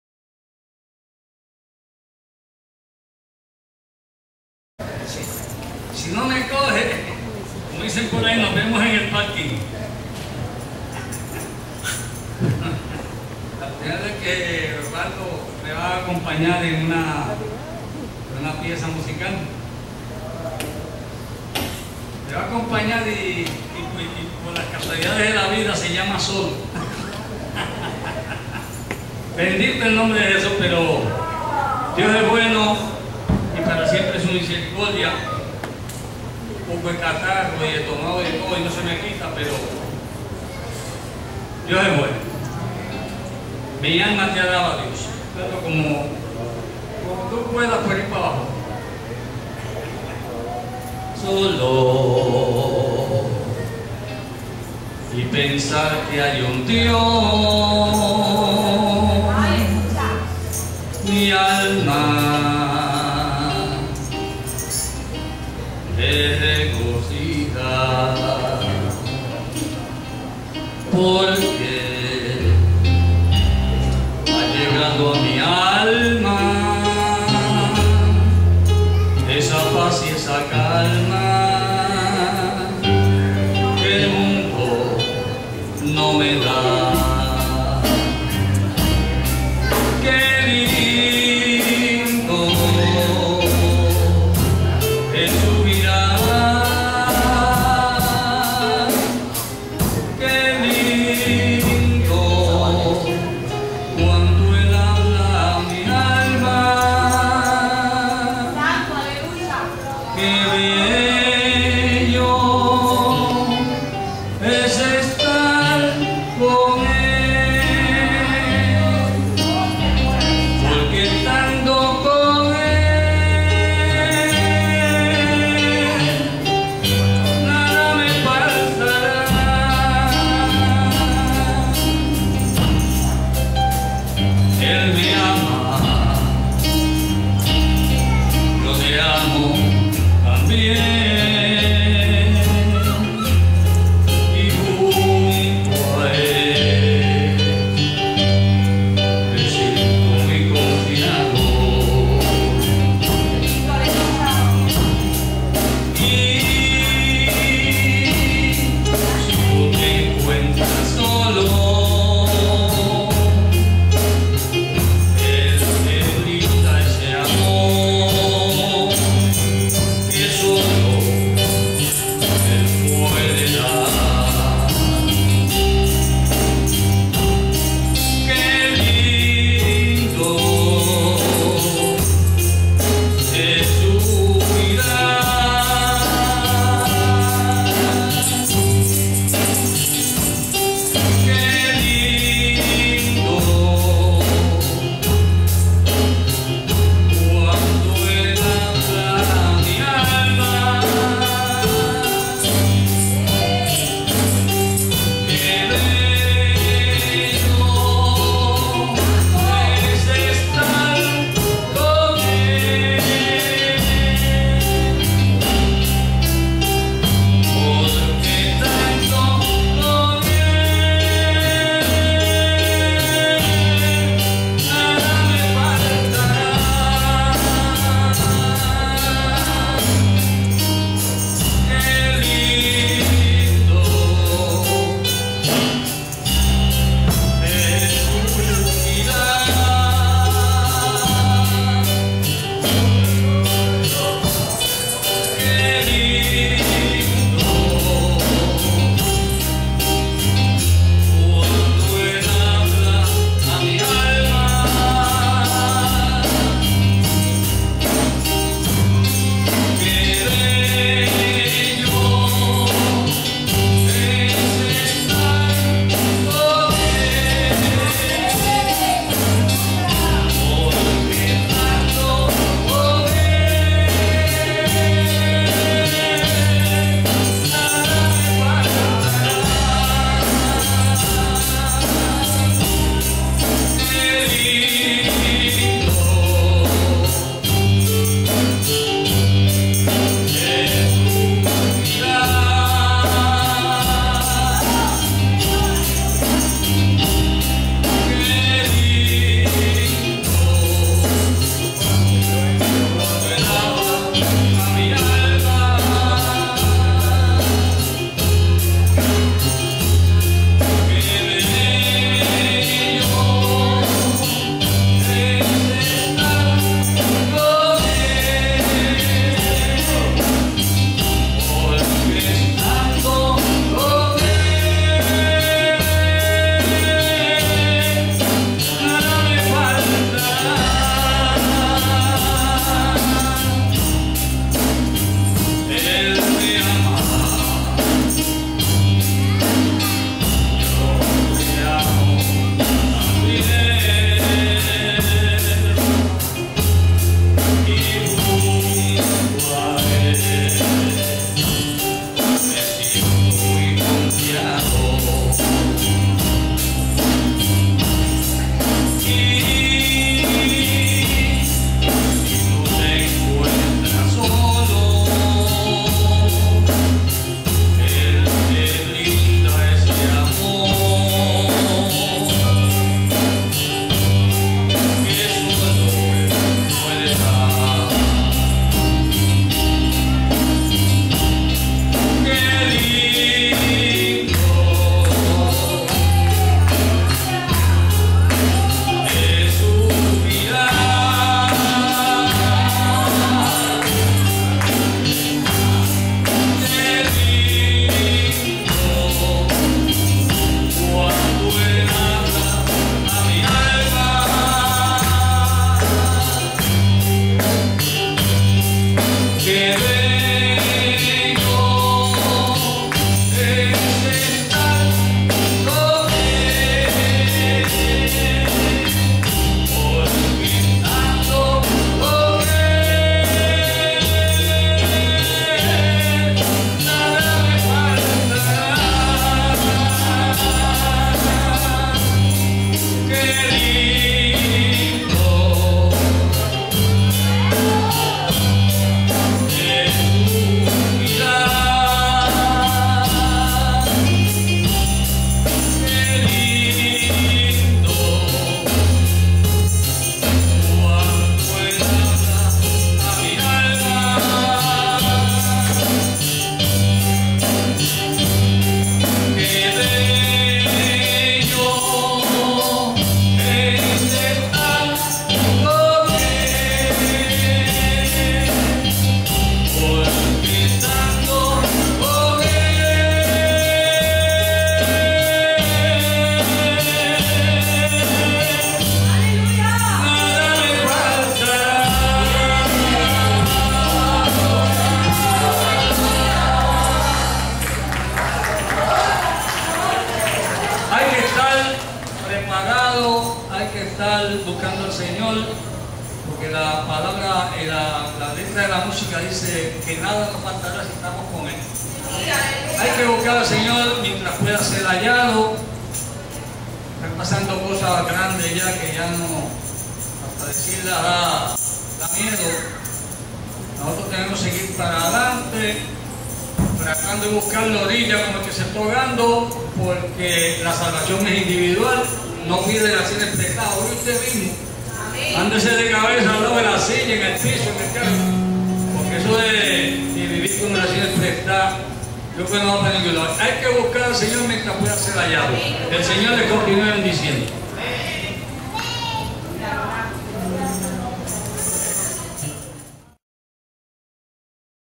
EL HNO.SHEGRAUD
EN VÍVO DESDE LA IGLESÍA JOVENES CRÍSTÍANOS DEL RES.VÍSTA HERMOSA QUE PASTOREA MÍ PASTOR FRANCÍSCO CABEZUDO,EL HNO.SHEGRAUD CON TODA LA POTENCÍA DE DÍOS.C.E.E.Q.M.DOMÍNGO 04 DE MARZO 2012.